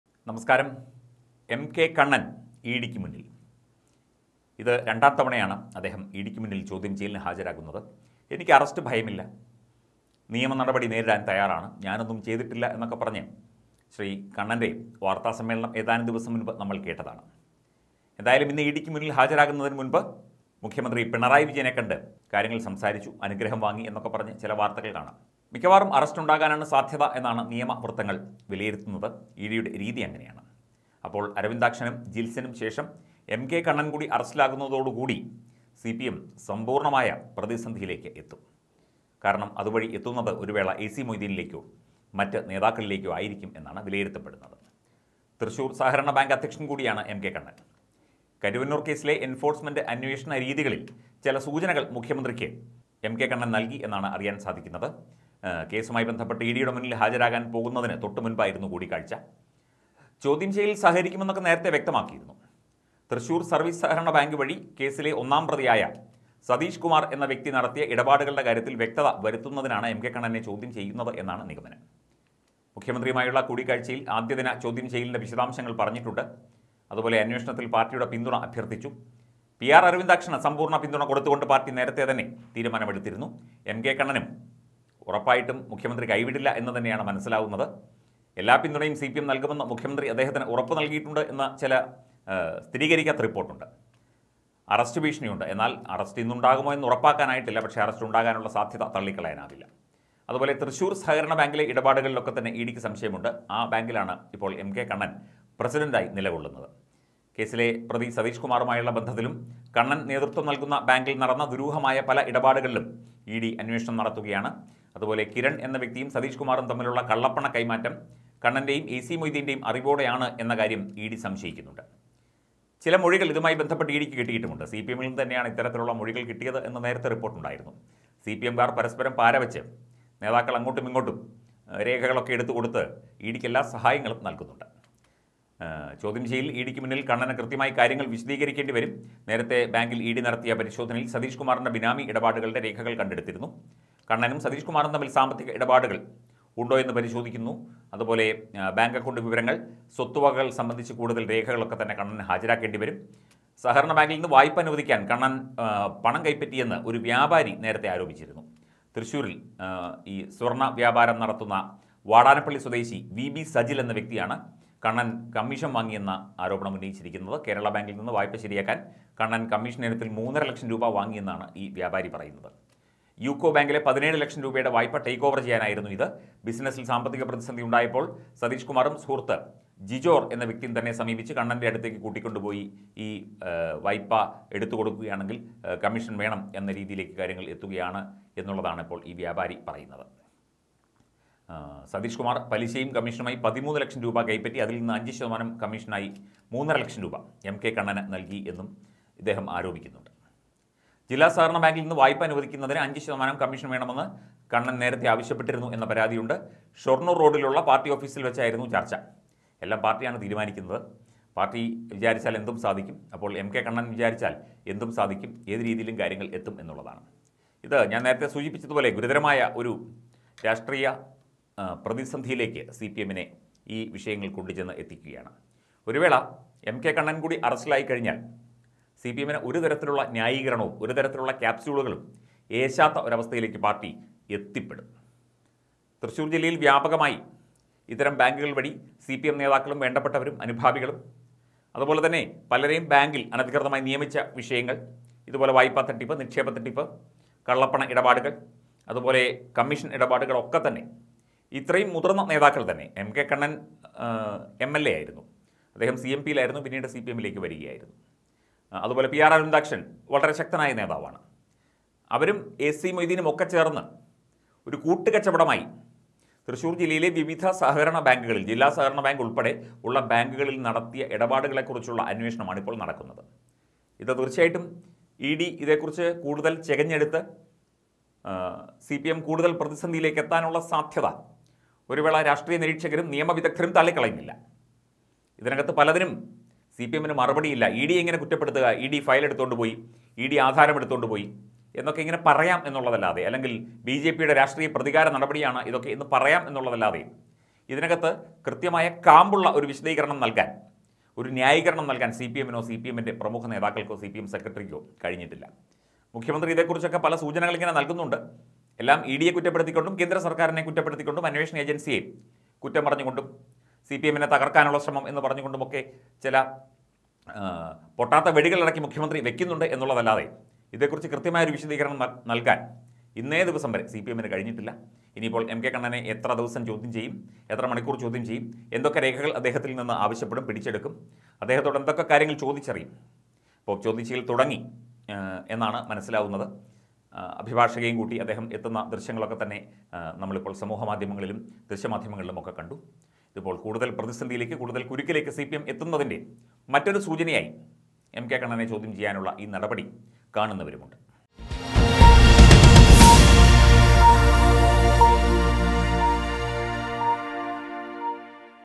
അറസ്റ്റ് ഭയന്ന് വിറയലോടെ എം കെ കണ്ണൻ | M.K.Kannan | Enforcement Directorate
നിറകണ്ണുകളോടെ,പിണറായിയുടെ ചെവിയിൽ സ്വകാര്യം പറഞ്ഞു
കണ്ണൻ ഇ ഡി ക്ക് മുന്നിൽ